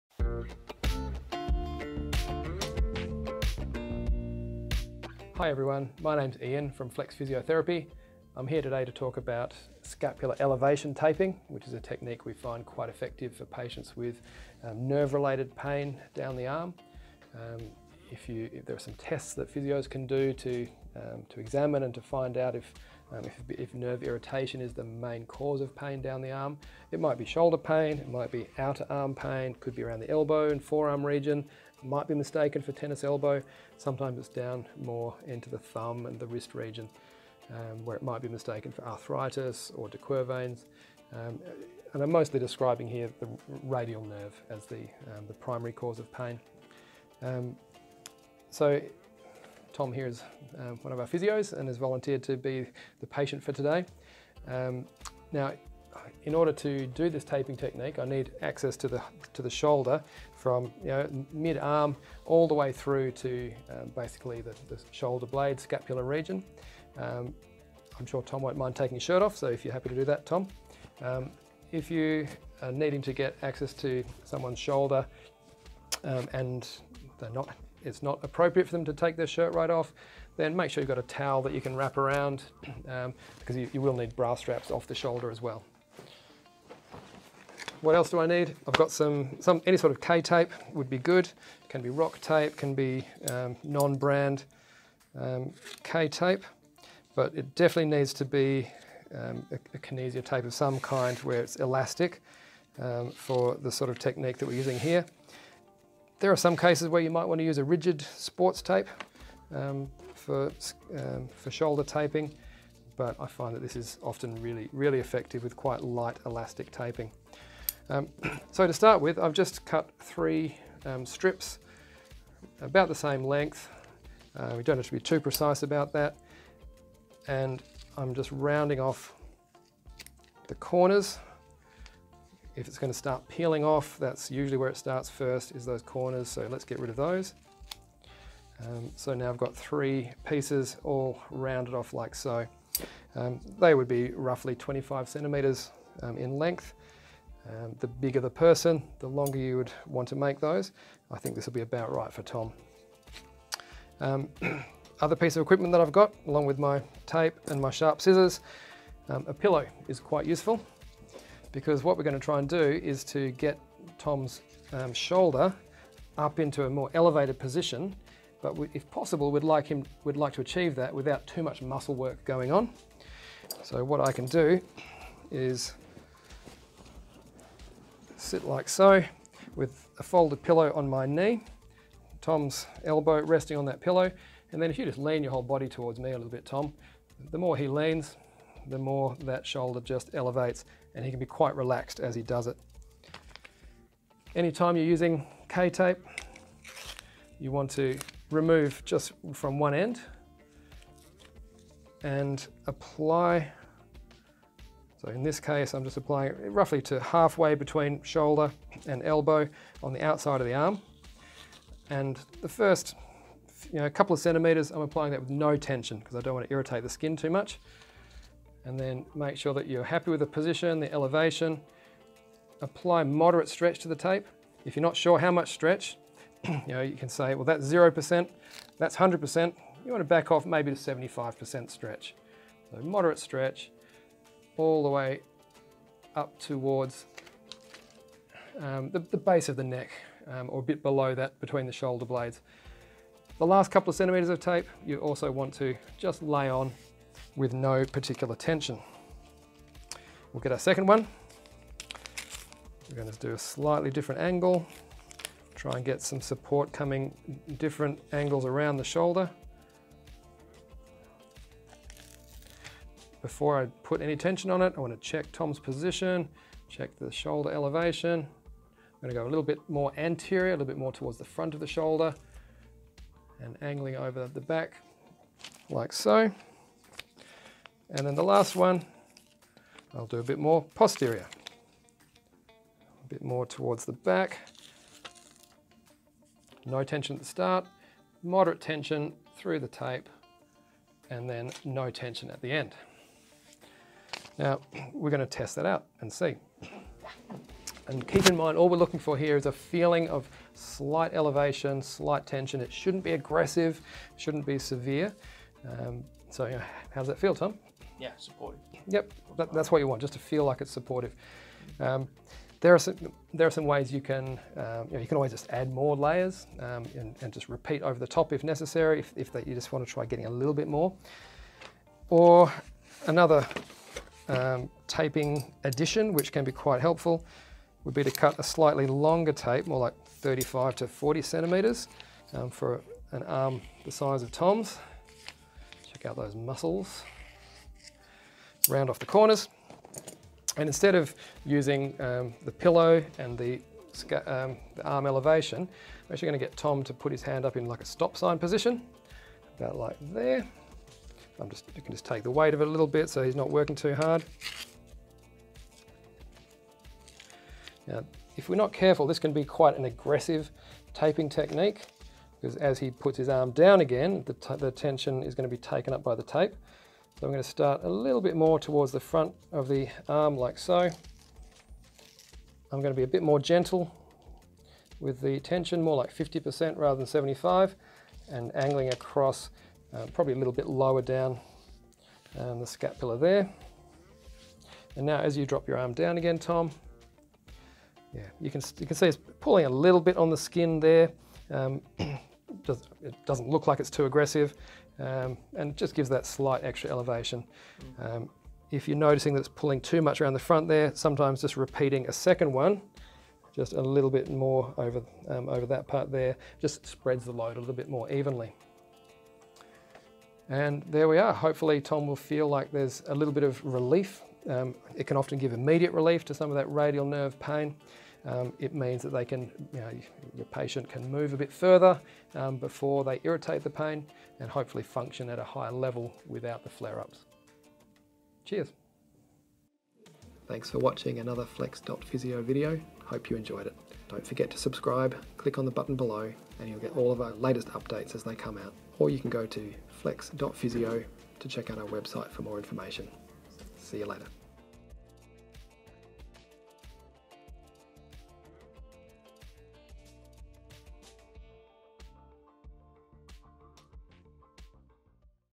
Scapula Taping - Radial Nerve
The Flex.Physio team show you how to scapula tape in this step by step taping tutorial

We now offer online video consultations, click the link to book yours today